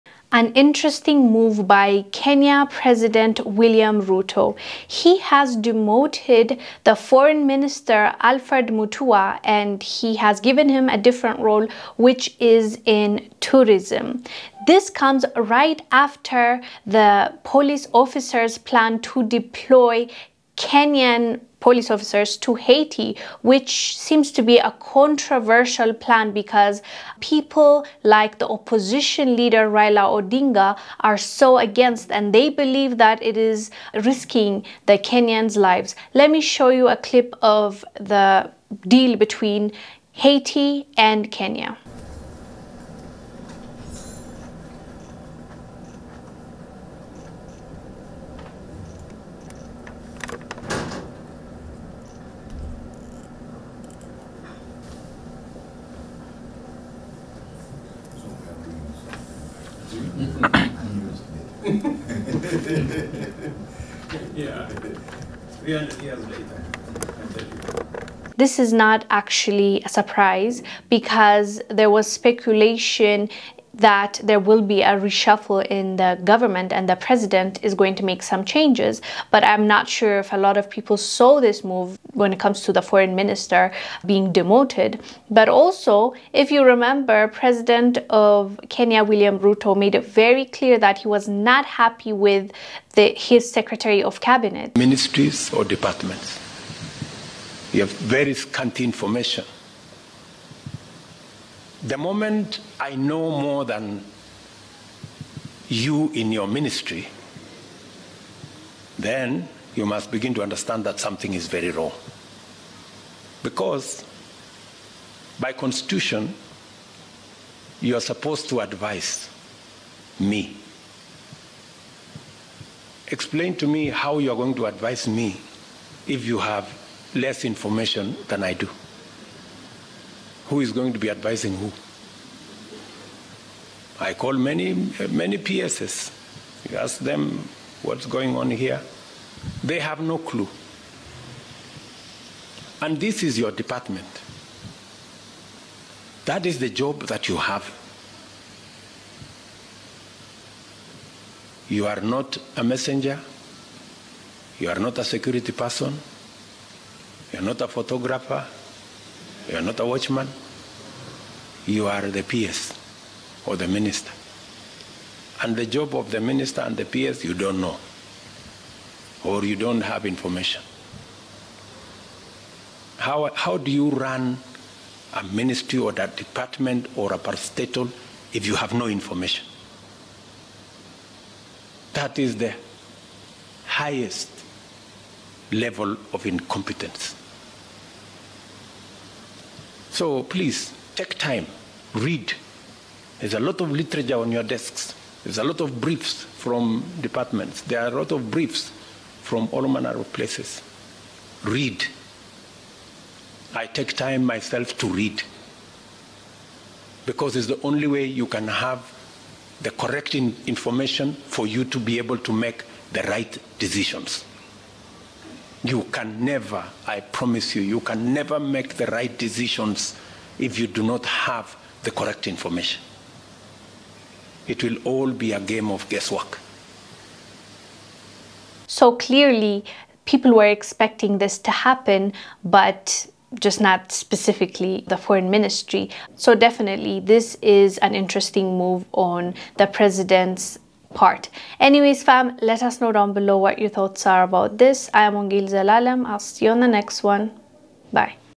Foreign Minister Alfred Demoted After Controversial Move To Deploy Kenyan Police To Haiti
Wongel Zelalem reports on Foreign Minister Alfred getting demoted After controversial move to deploy Kenyan Police to Haiti.